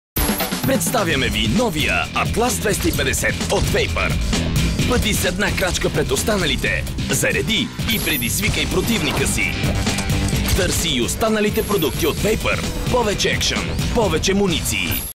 Vapor от Smartshopping.bg
Страхотната  нова серия от Vapor е вече тук ! Бъдете с една крачка пред останалите -- зареди и  предизвикай противника си! Благодарение на гел топчетата вие може да  стреляте 50 пъти без да презареждате и с точност до 18 метра!